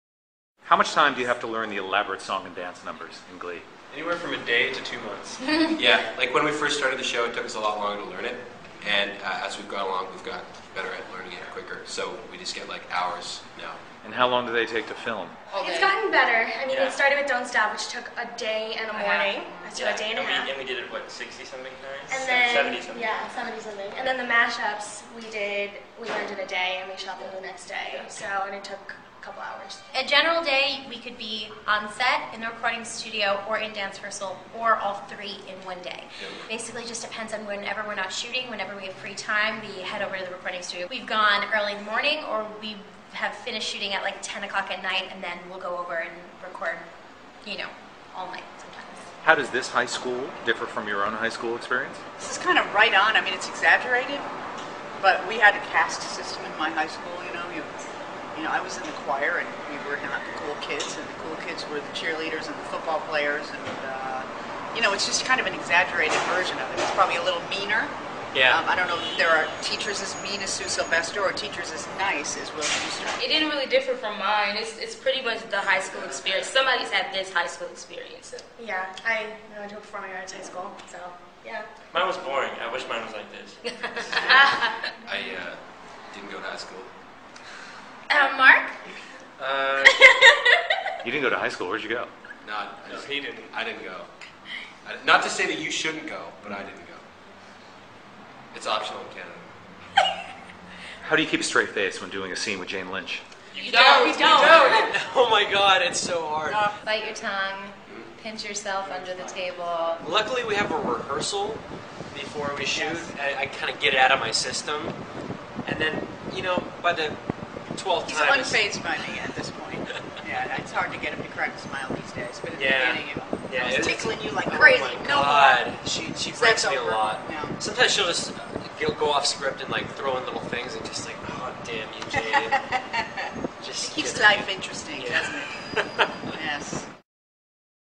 Glee- Entertainment Weekly Photo Shoot
The cast of Glee during Entertainment Weekly's photo shoot.